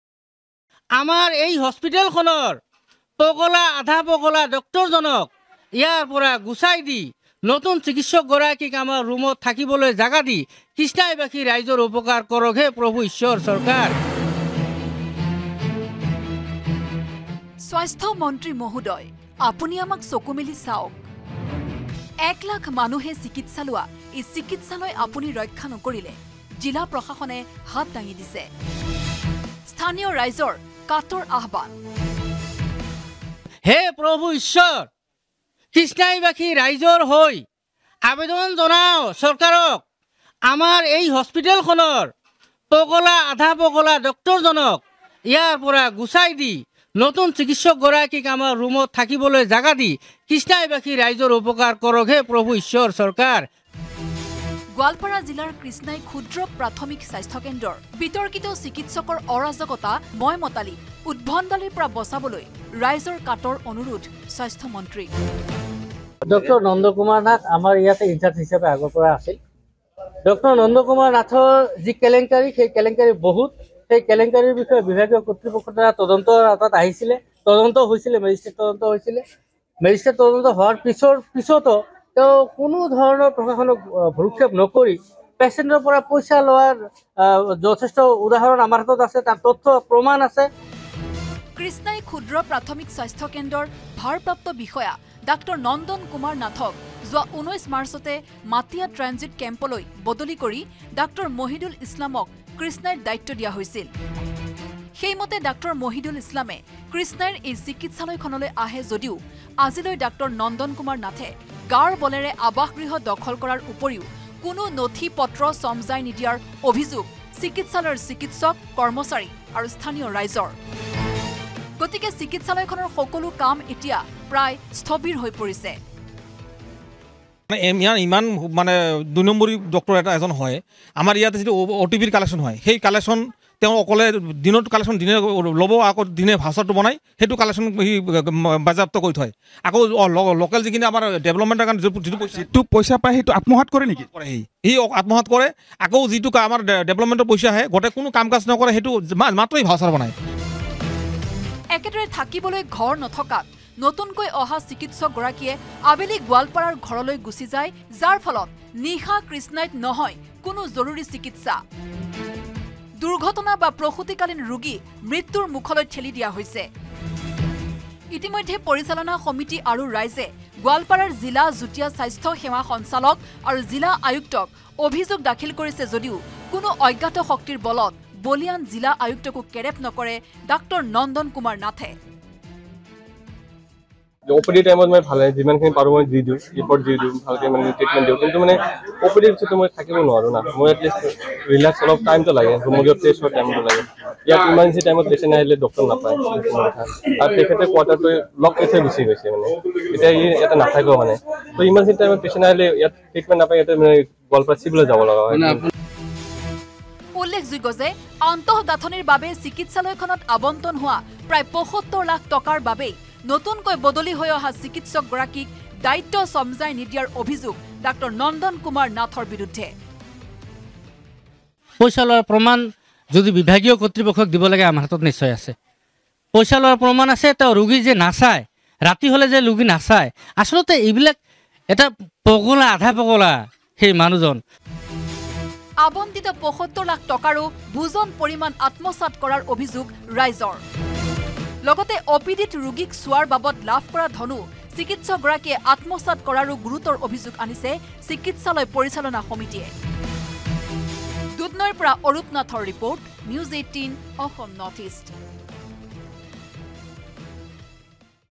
Goalpara News | ডেমকেয়াৰ ডাক্তৰ নন্দন কুমাৰ নাথক লৈ গোৱালপাৰাৰ কৃষ্ণাইত উত্তেজনা N18V
ডেমকেয়াৰ ডাক্তৰ নন্দন কুমাৰ নাথক লৈ গোৱালপাৰাৰ কৃষ্ণাইত উত্তেজনা। বদলিৰ নিৰ্দেশ ওলোৱাৰ ৪০ দিনৰ পিছতো ডাক্তৰ নন্দন কুমাৰ নাথে চিকিৎসালয় নেৰাত ক্ষোভতি হৈ পৰিছে কৃষ্ণাইৰ জনতা।